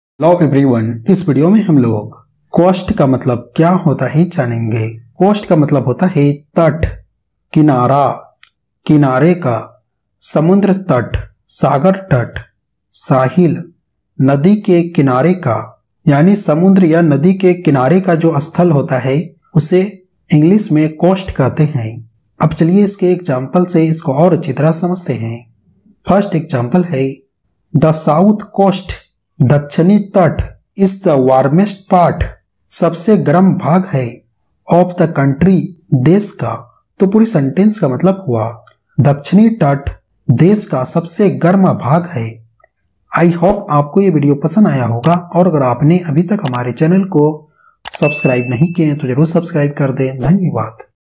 Coast Meaning in Hindi | Coast ka Matlab kya hota hai | Word Meaning English to Hindi
Coast Meaning in Hindi | Meaning of Coast | Coast का हिंदी में मतलब | English to Hindi vocabulary |

Aaj ki Is Video Me Maine Aapko Coast Ka Matlab Kya hota hai Hindi mein, Coast Ka definition or Coast Word Ke Sentence ka Example ko batlaya hai.